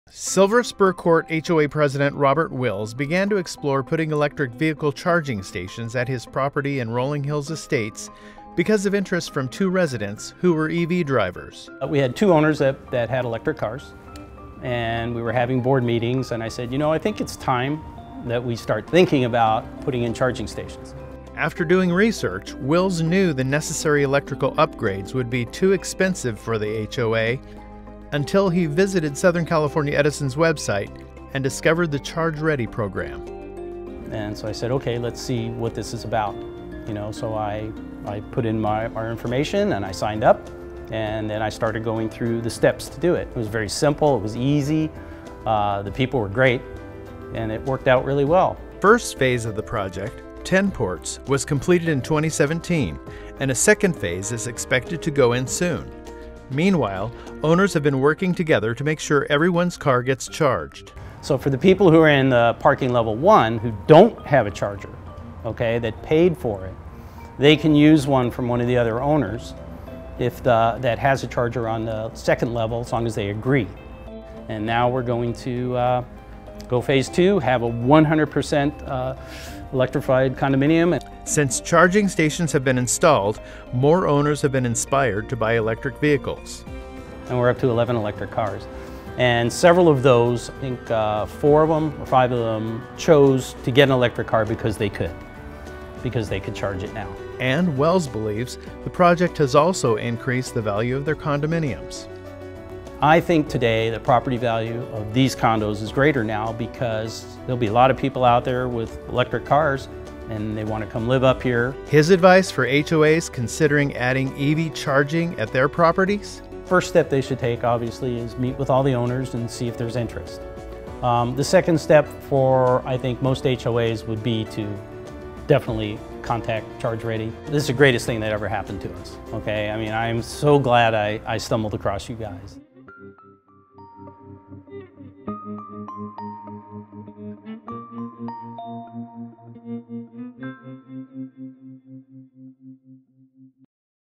Exploring EV Charging at Silver Spur Court | SCE Charge Ready Program
HOA president, Robert Wills, shares how Silver Spur Court Condominium residents worked together to develop creative solutions and leverage Southern California Edison's (SCE) Charge Ready program.